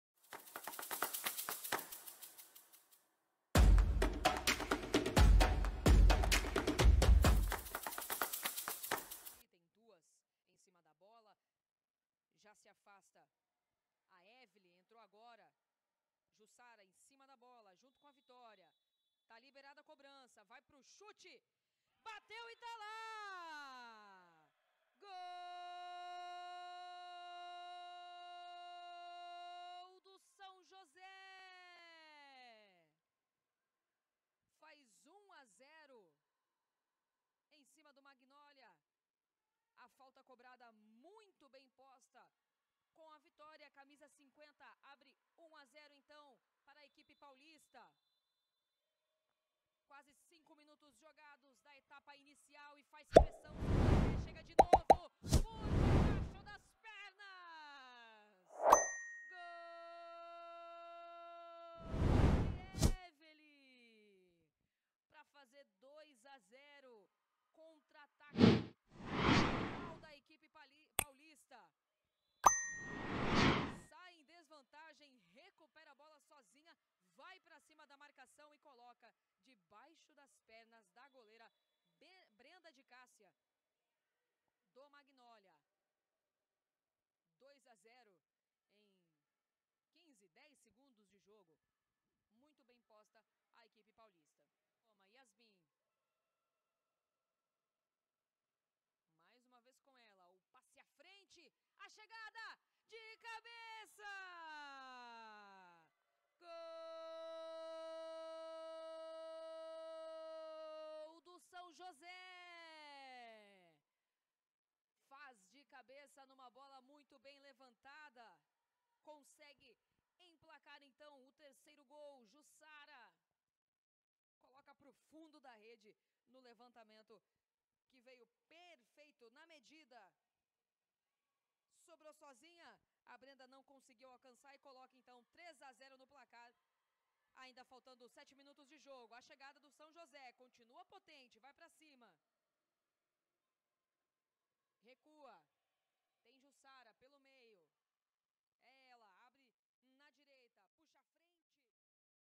GOLS | MAGNOLIA CAD 0 x 3 SÃO JOSÉ | TAÇA BRASIL DE FUTSAL FEMININO 2022
TAÇA BRASIL DE FUTSAL FEMININO 2022

MAGNOLIA CAD 0 x 3 SÃO JOSÉ

GINÁSIO GUANANDIZÃO

IMAGENS: TARGET TV / CBFS